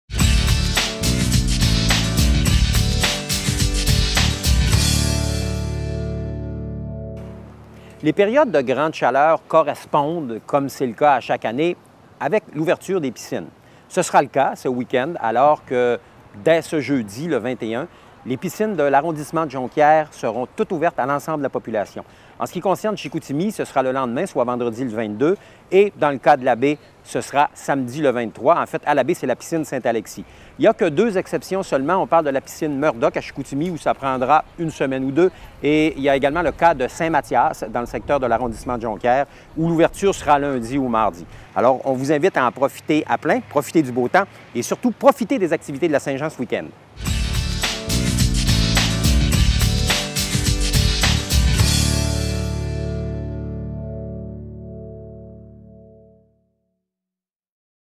Saguenay ouvre ses piscines extérieures
Plusieurs seront heureux d'apprendre que Saguenay procède à l'ouverture de ses piscines extérieures. Le directeur du service des communications de la ville Jeannot Allard, nous donne plus de détails.